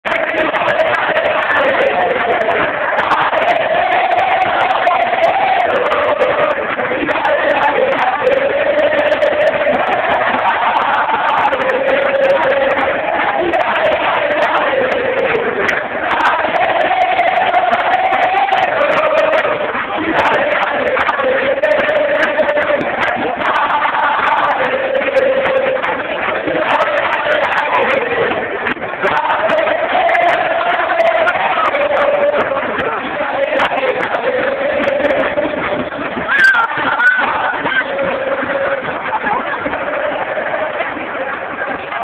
COLON DE SANTA FE - Fiesta Sabalera
Colon 5 vs 1 TIgre - Apertura 09.  Dale negro dale negro y dale dale dale neee, dale nee, y dale dale dale negro